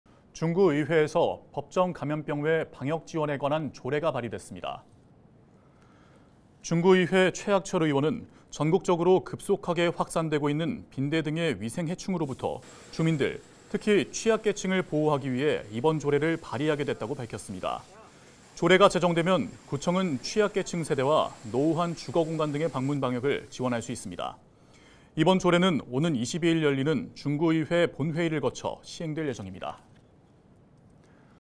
중구의회 최학철 의원, '빈대' 방역 지원 조례 발의ㅣ#헬로tv뉴스
중구의회 최학철 의원은 전국적으로 급속하게 확산하고 있는 빈대 등의 위생해충으로부터 주민들, 특히 취약계층을 보호하기 위해 이번 조례를 발의하게 됐다고 밝혔습니다.
📌 카카오톡 ID : 헬로tv뉴스